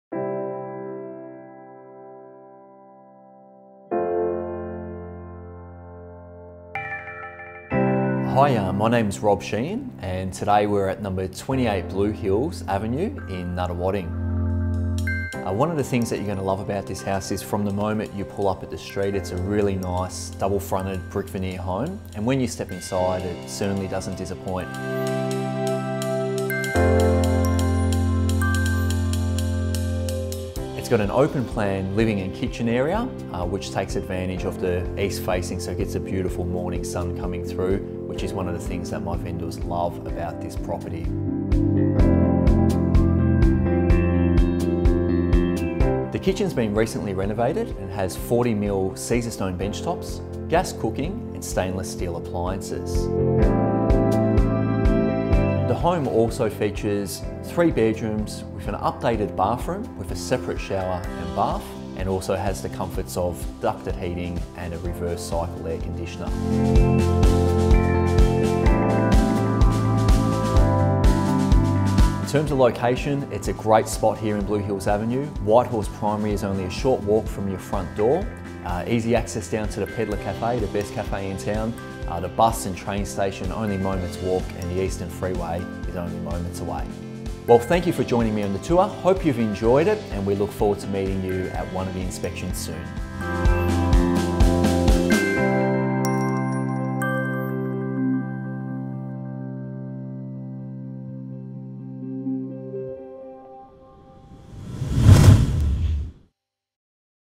Fletchers - 28 Blue Hills Avenue, Nunawading - Robert Sheahan
Nestled in a great family locale and awaiting a fresh set of happy memories, enjoy the easy stroll to Whitehorse and St Philip’s Primary Schools, delightful cafes, Blackburn North Shopping Centre and Blackburn High School from this sun-filled home.

With glistening polished boards flowing from the moment you step inside, journey through to the enticing lounge and dining room to the brand new kitchen with stone benches, glass splashbacks and stainless steel appliances.

Relax out in the peaceful alfresco and level garden with covered west-facing porch for peaceful afternoons in the sunshine.

The 3 bedrooms are accompanied by a sparkling bathroom, while other features include gas ducted heating, air-conditioner, security, single carport and single garage. Near the Eastern Freeway, bus and train.